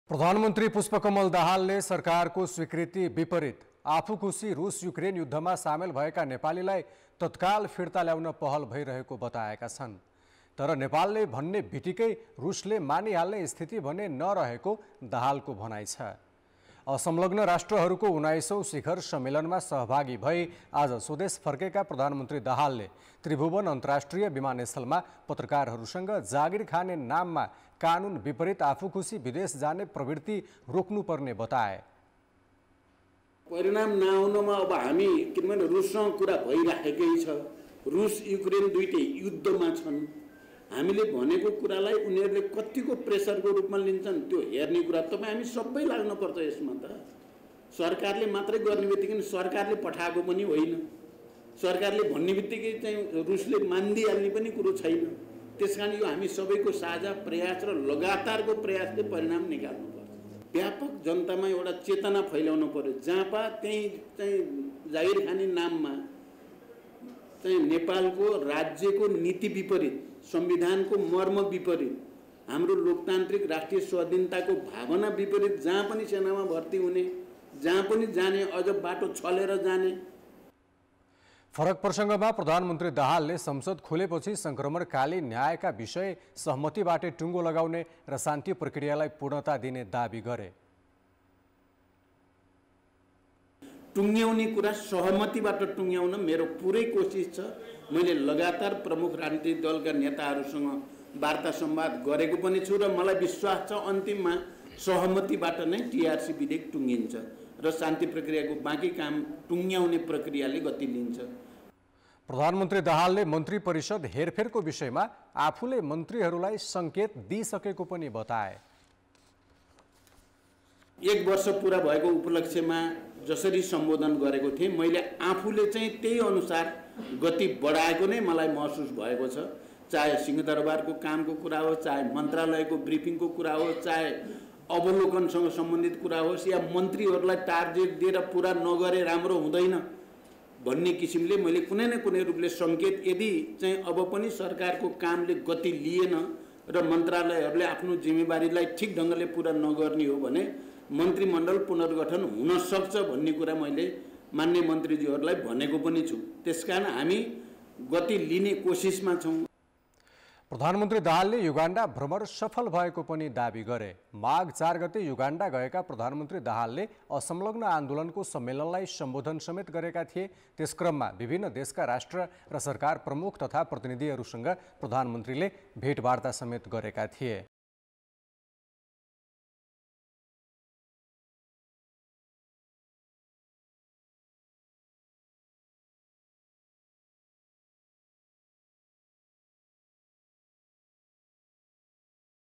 रुस-युक्रेन युद्धमा नेपाली : प्रधानमन्त्री दाहालको अशोभनीय अभिव्यक्ति | Kantipur Samachar
प्रधानमन्त्री पुष्पकमल दाहालले सरकारको स्वीकृतिविपरीत आफूखुसी रुस-युक्रेन युद्धमा सामेल भएका नेपालीलाई फिर्ता तत्काल फिर्ता ल्याउन पहल भैरहेको बताएका छन् । तर नेपालले भन्ने बित्तिकै रुसले मानिहाल्ने स्थिति भने नरहेको दाहालको भनाइ छ ।